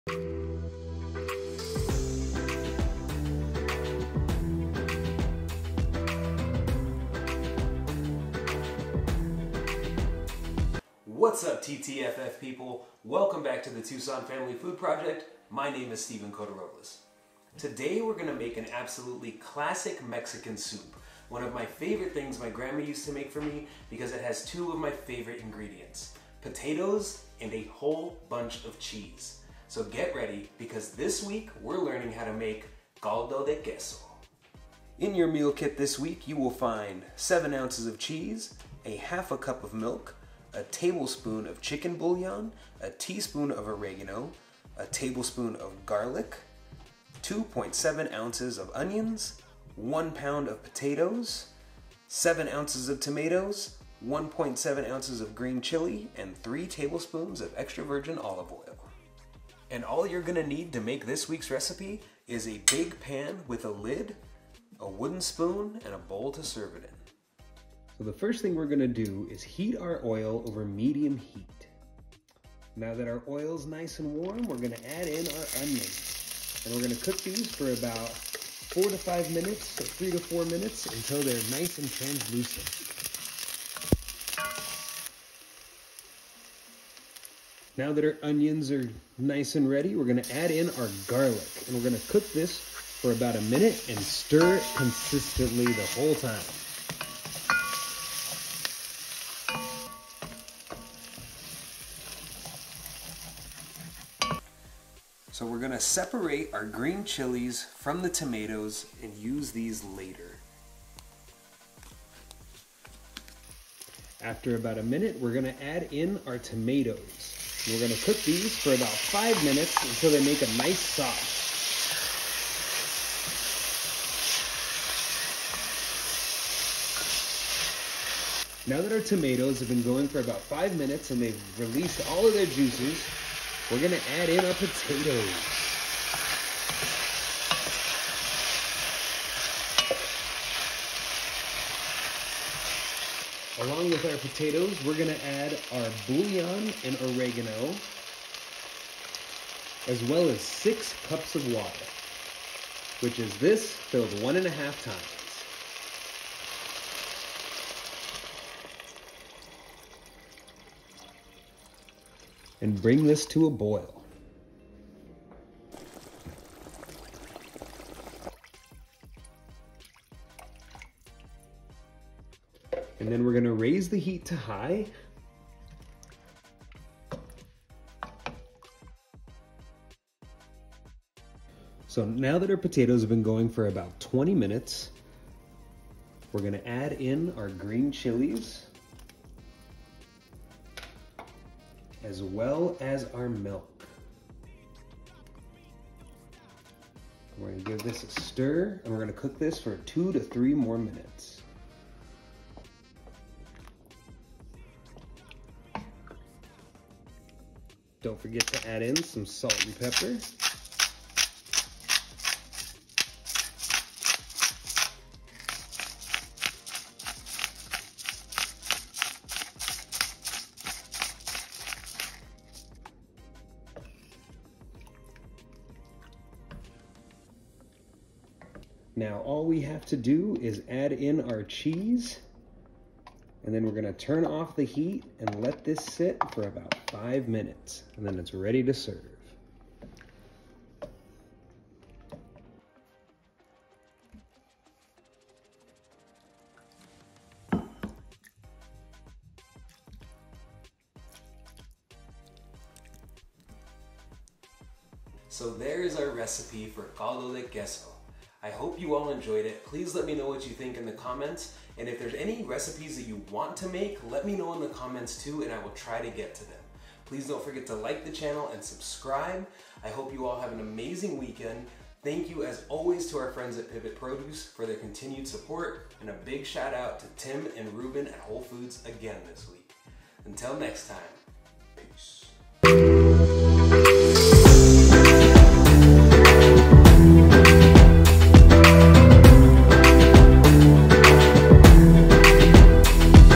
Caldo de Queso
Ingredients
Potatoes – 1lb   Tomaotes – 7oz  Cheese – 7oz  Onion – 2.7oz
EVOO - 3 tbsp  Milk – ½ cup   Green Chili – 1.7oz  Garlic – ½ tbsp
Oregano – 1 tsp  Chicken bouillon – 1 tbsp
Equipment
Large pot w/lid    Wooden spoon
Instructions:
1)Wash your hands
2)In a large pot, heat the oil over medium heat and add the onion. Sautee for 3-4 minutes or until the onion is translucent.
3)Add in your garlic. Cook and stir for about 1 minute.
4)Remove the chilies from the tomatoes and set aside. Then add in your tomatoes to the pan and cook for about 5 minutes or until you start to form a nice sauce.
5)Drain your potatoes and add them to the pot along with 6 cups of water and the bouillon and oregano and bring to a boil
(6 cups is the potato container filled 1 and ½ times)
6)Once the soup is boiling, cover with a lid, lower heat to low and let cook for about 20-25 minutes or until the potatoes are tender
7)Once the potatoes are tender, add your green chilies and milk to the pot. Mix and cook for 2-3 more minutes.
8)Turn off the heat and immediately add the cheese. Mix and let stand for about 5 minutes
9)Serve and enjoy!